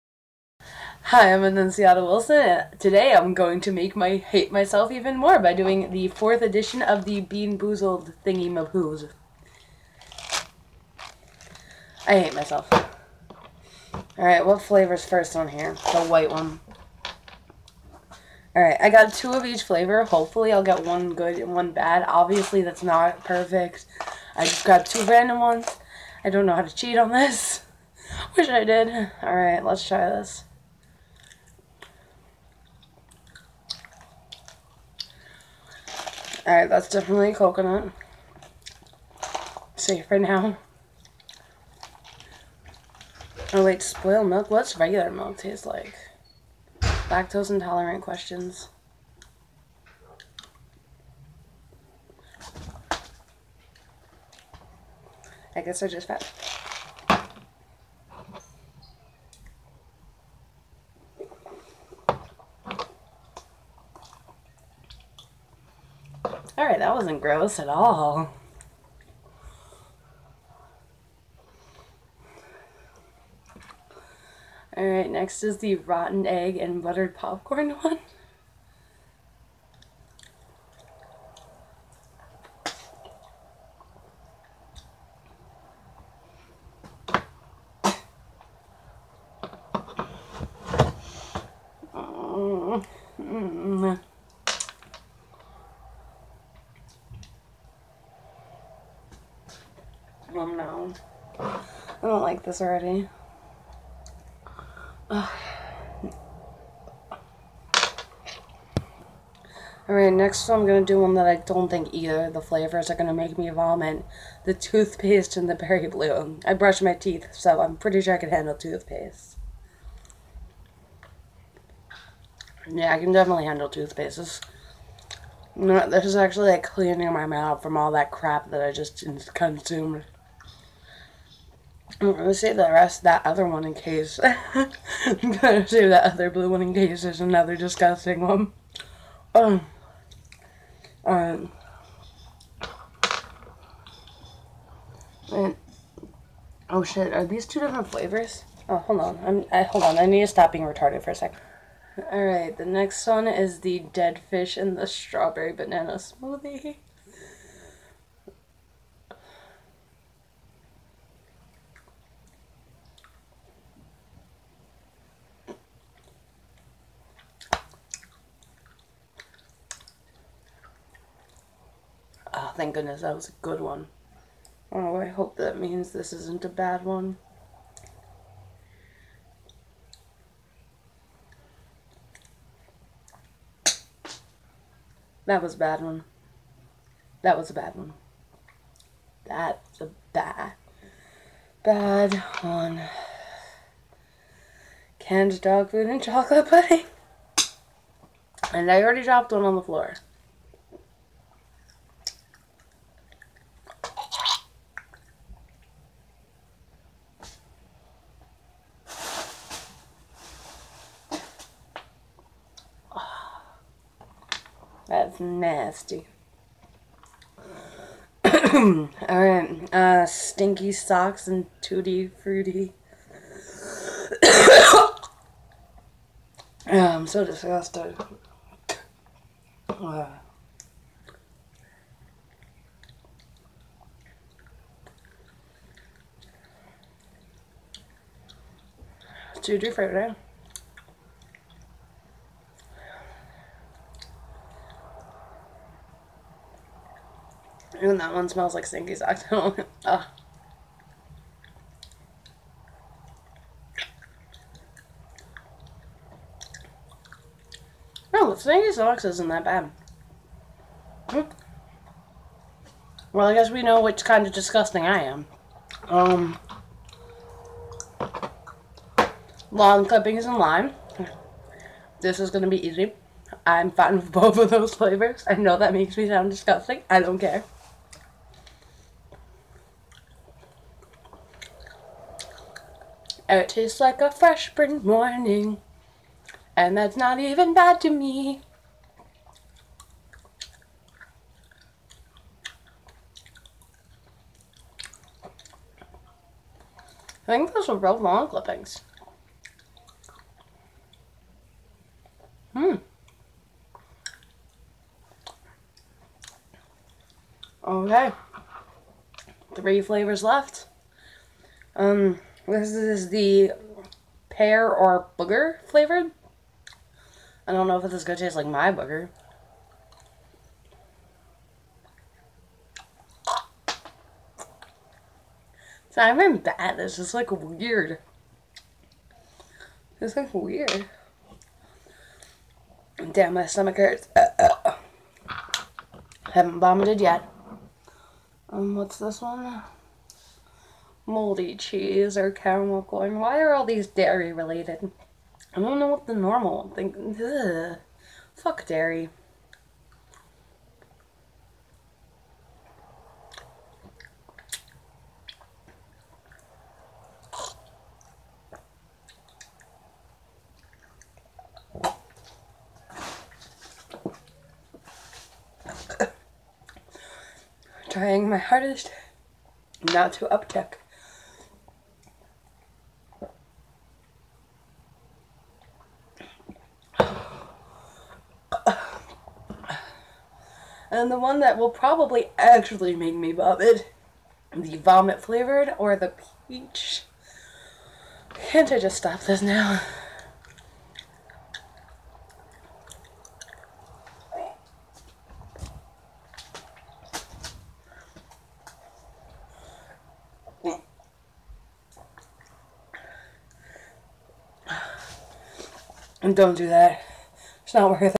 Bean Boozled Challenge
Shit, I though I was going to vomit on camera.Kind of wish I did, for the views. Never doing that again in my life.